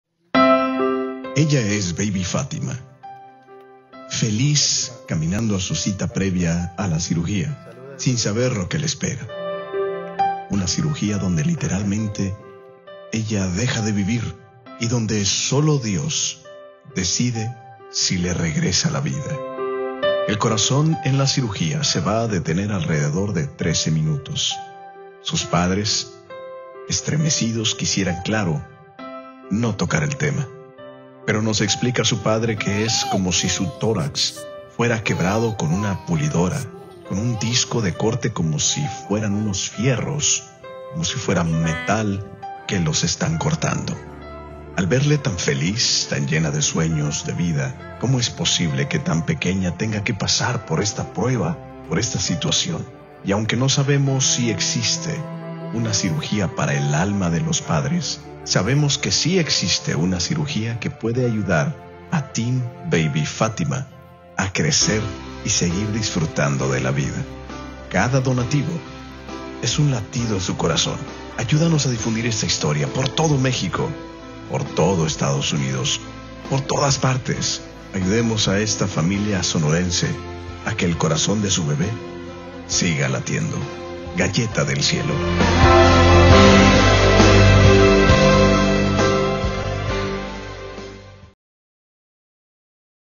Su Corazoncito esta en riesgo 😞🥺
La vida de BabyFATIMA depende de 3 millones de pesos mexicanos. 😞💔

✅ En México:

POR FAVOR ayudanos a hacer viral esta historia.